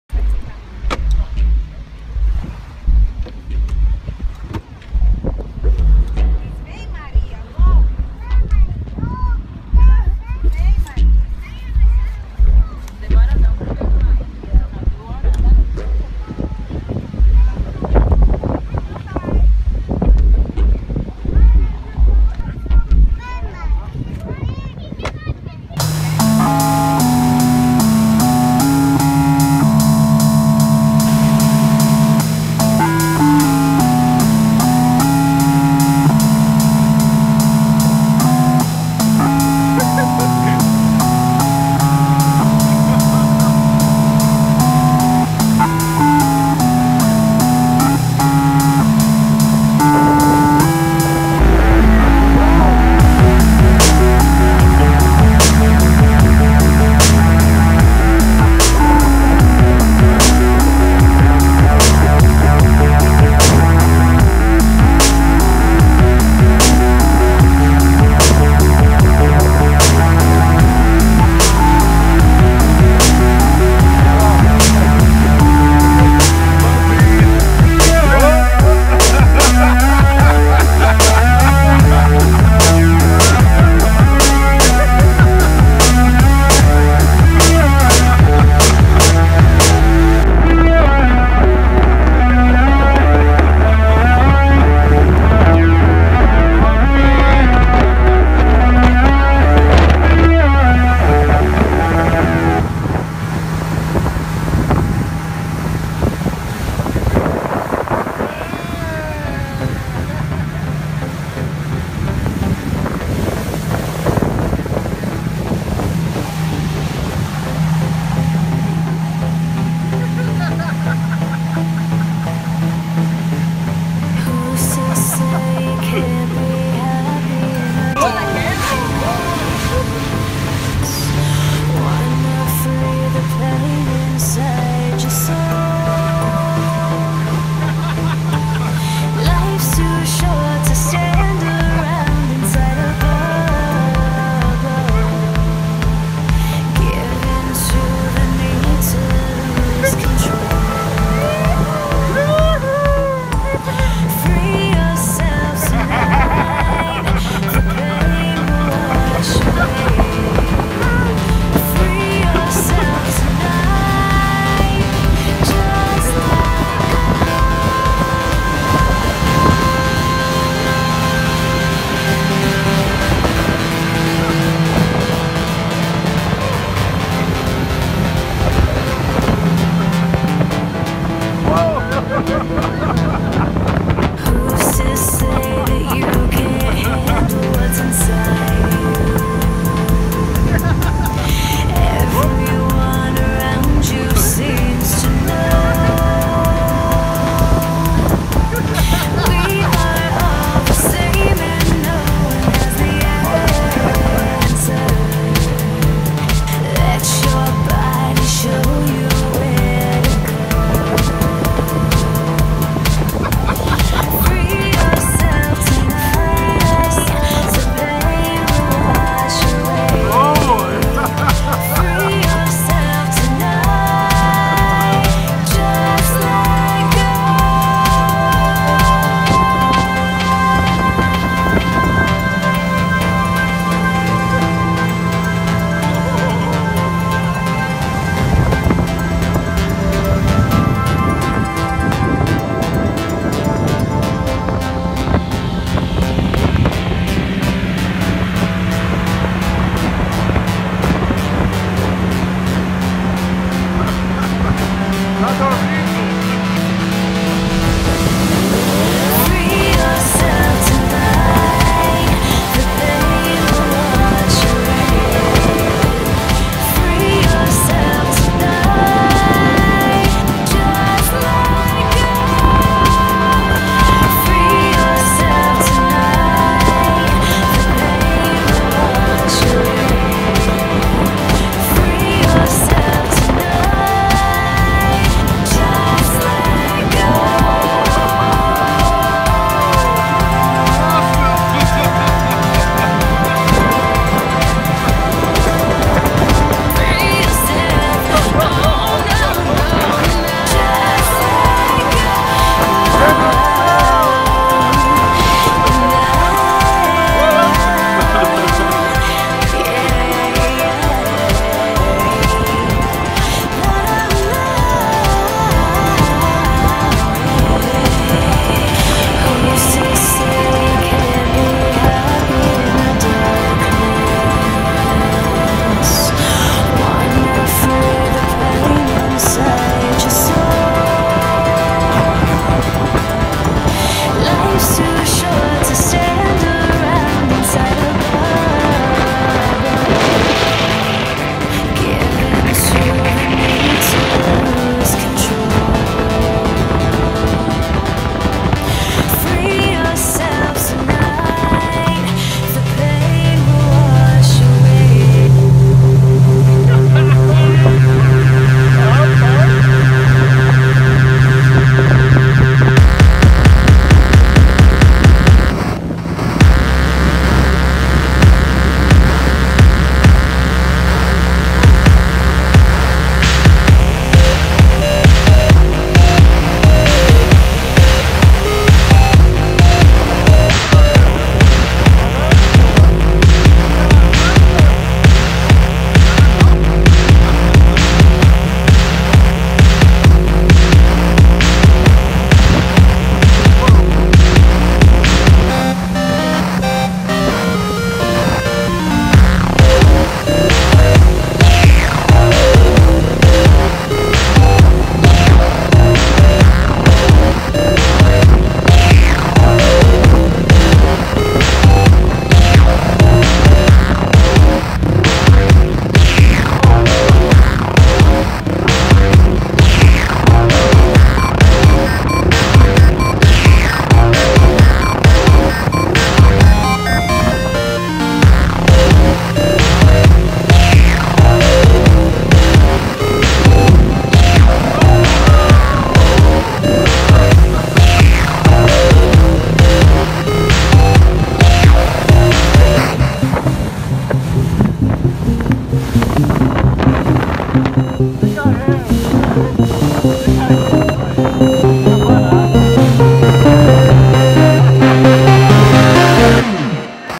Short Fun Boating Adventure
Went with friends and family to have some fun on a lake in Idaho. It was an amazingly beautiful day as you will see and very fun.